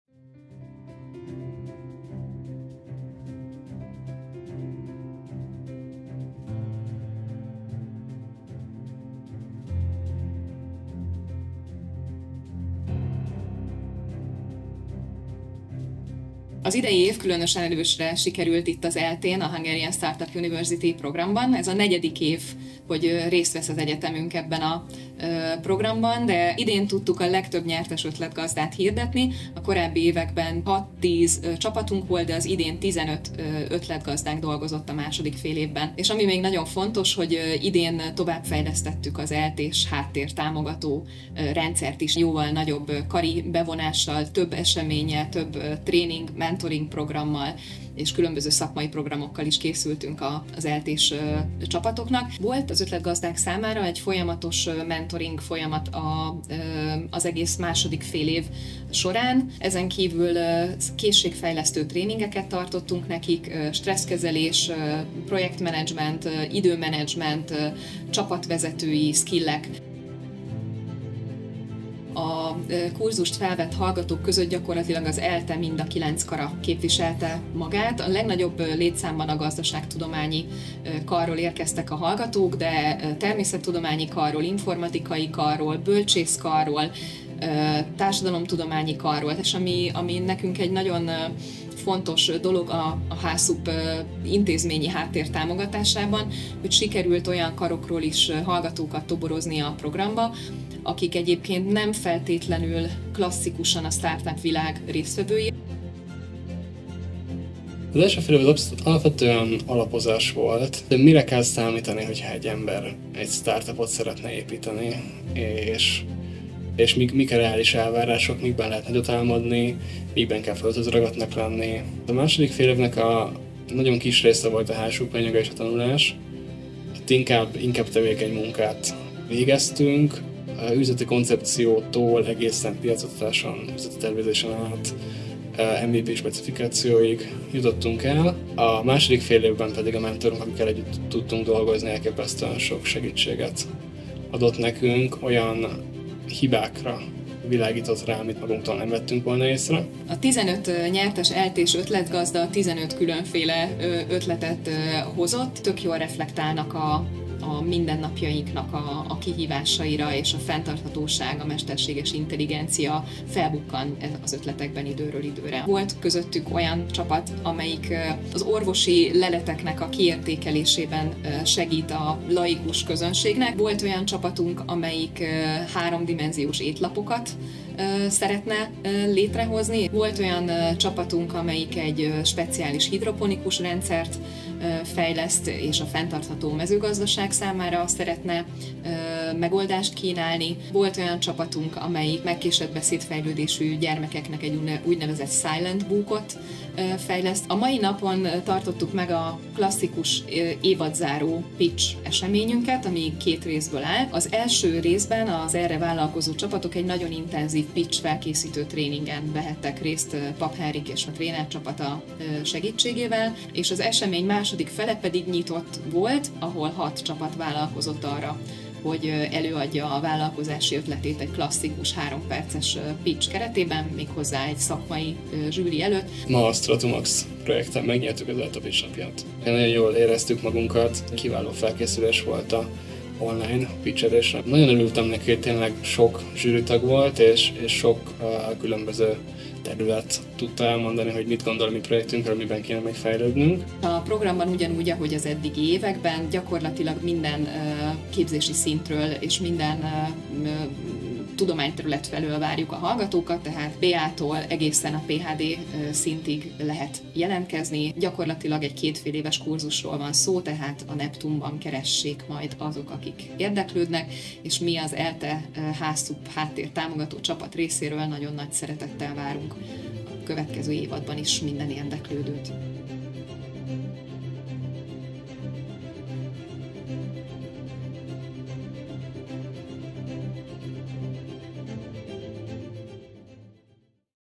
A Hungarian Startup University Program az ELTE-n (2023/2024)
Eredményes évet zárt a Hungarian Startup University Program az Eötvös Loránd Tudományegyetemen. Rekordszámú, közel 290 hallgató vett részt a program idei évadában. Az ELTE HSUP háttércsapata személyre szabott mentorálást és a startup-világból érkező előadókkal szervezett szakmai programokat kínált az érdeklődő hallgatóknak a tanév során, valamint általános készségfejlesztő, például az időmenedzsmentet, a stresszkezelést és a csapatvezetést támogató workshopok is várták őket. Interjúnkban az idei évről Szöllősi Szilvia intézményi HSUP-koordinátor, az ELTE Egyetemi Stratégiai Iroda vezetője és Gajdos Marcell hallgató nyilatkozik.